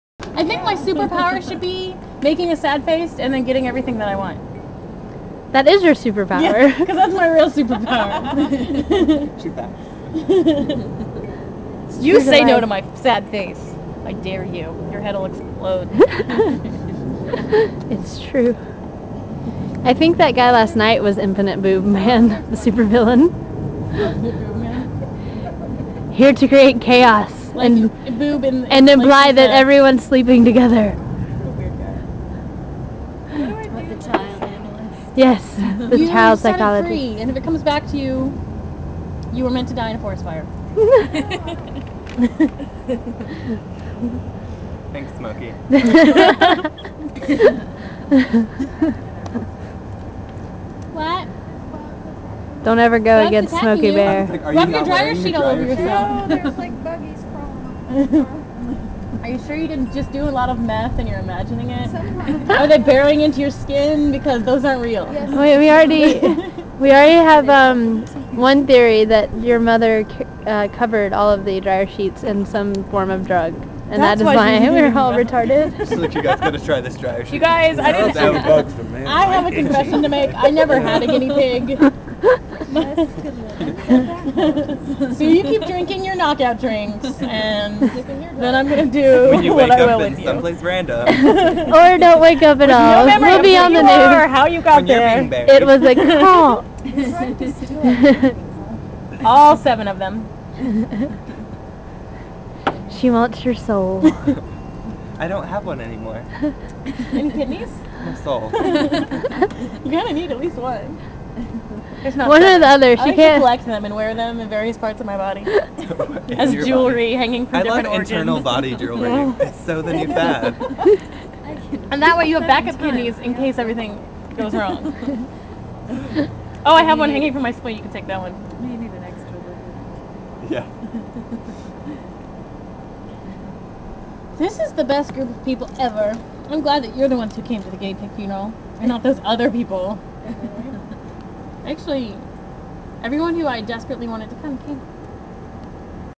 Gelato Wake: Superpowers
Making sad faces and getting everything you want as a super power. Drugged dryer sheets, knockout drinks, and internal body jewelry.